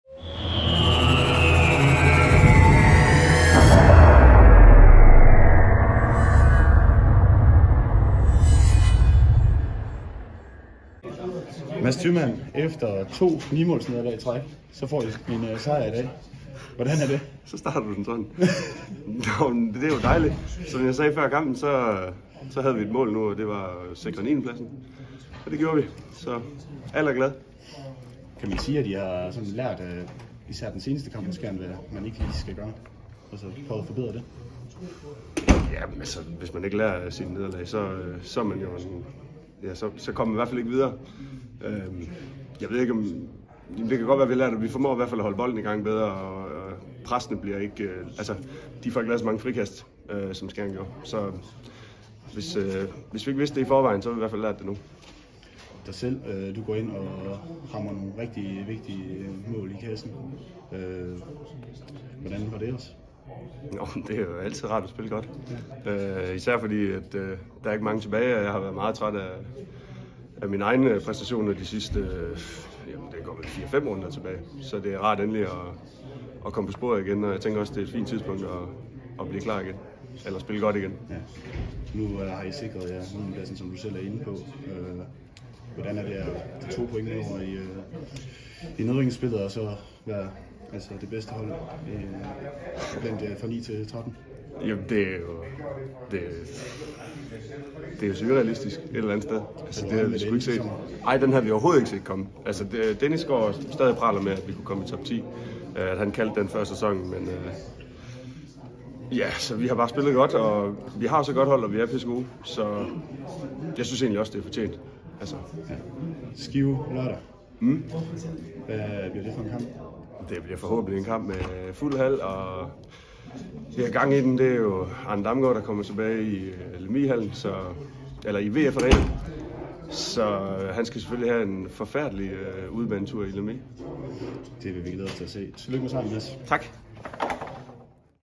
Mads Thymann efter Skanderborg Aarhus
Hør hvad Mads Thymann havde at sige efter sejren på udebane over Skanderborg Aarhus.